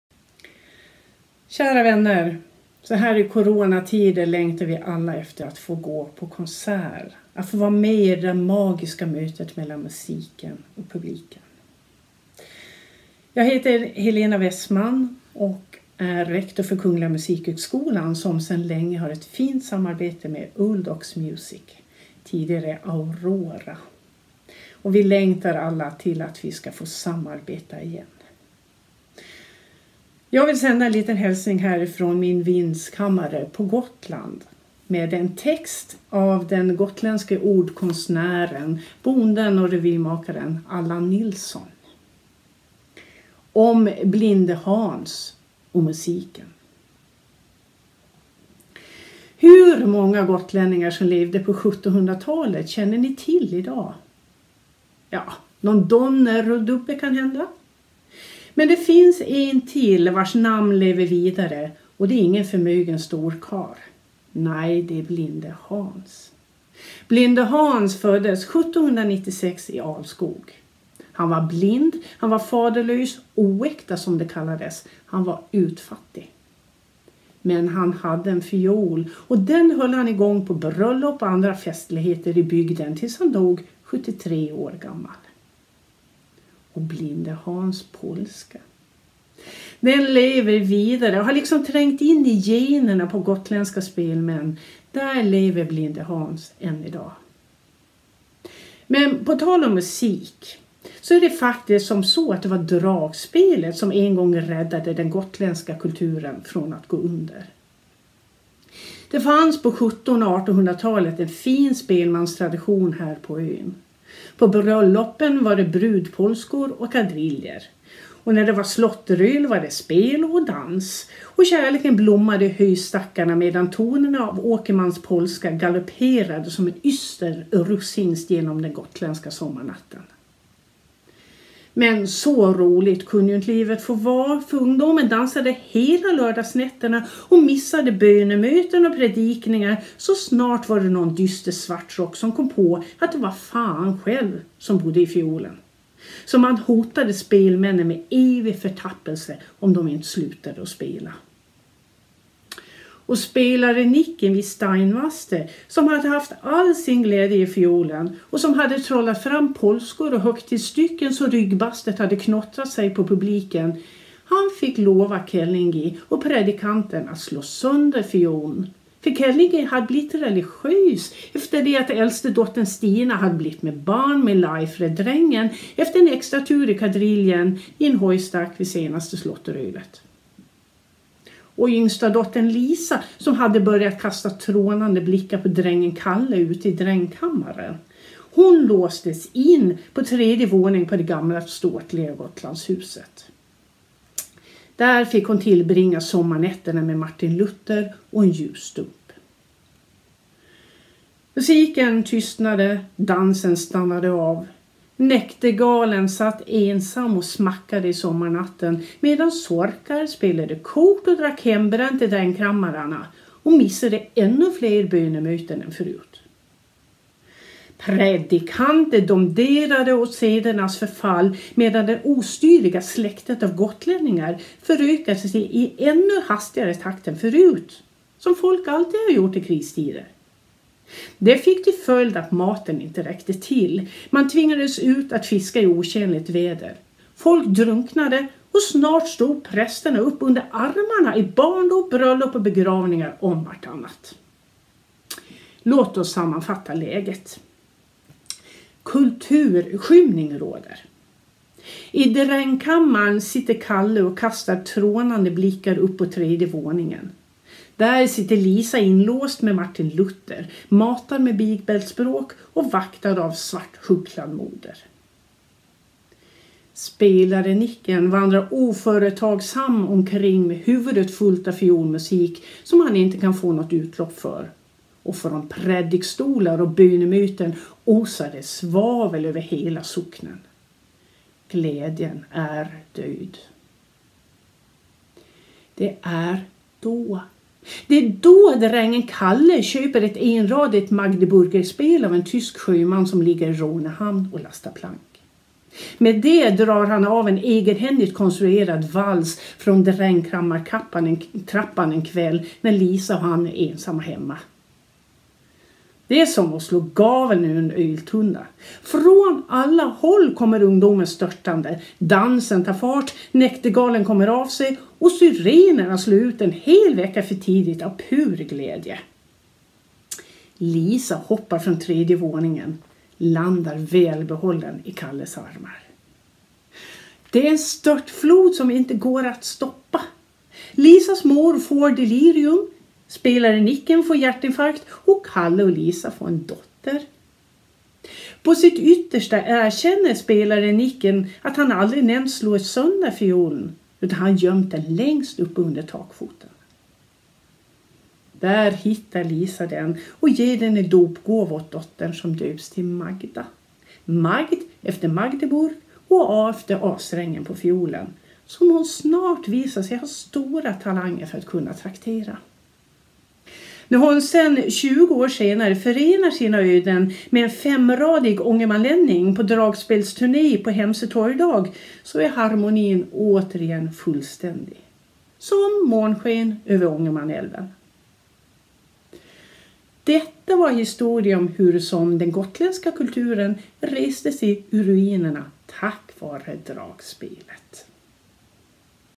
Helena Wessman - Längtar till musiken 7
Helena Wessman, rektor vid Kungliga Musikhögskolan i Stockholm, skickar hälsningar och skrönor från Gotland till Old Oxpubliken.

KMH och OLD OX har samarbetat i 14 år för att stärka den klassiska musiken i alla led.
Ett mycket framgångsrikt samarbete som också fått mycket stor internationell uppmärksamhet.